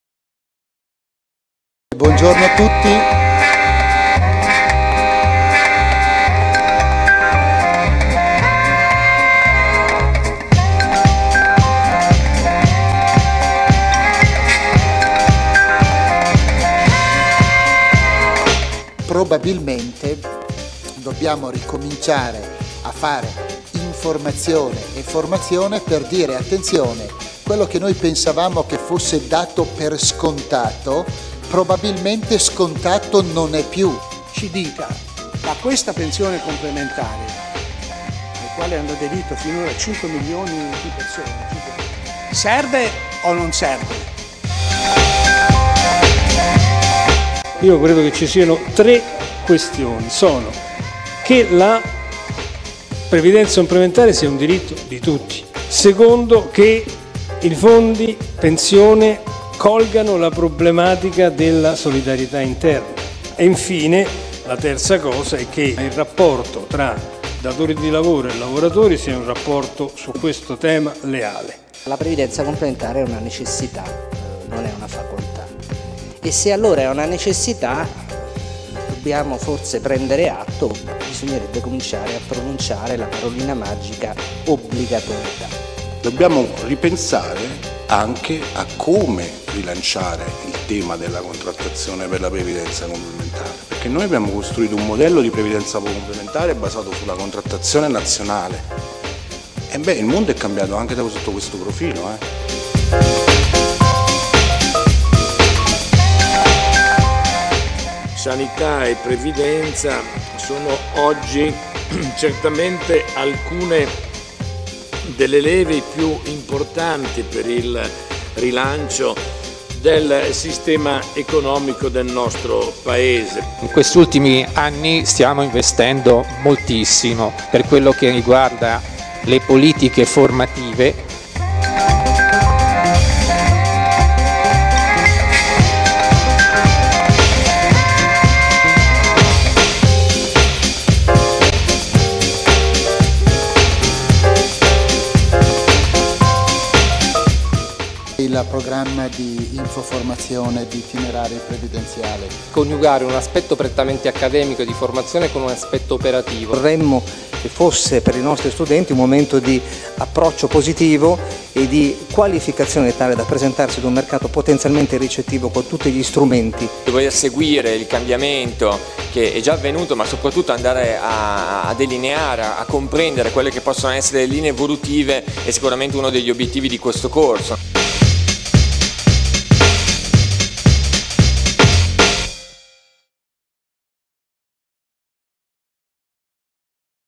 Convegno 24/10/2011 - Itinerari Previdenziali
INFORMAZIONE E RESPONSABILITA' PER AUMENTARE I TASSI DI ADESIONE AI FONDI PENSIONE:
Quali proposte per un rilancio della previdenza complementare?

Lunedì 24 Ottobre 2011 | Unione del Commercio
Milano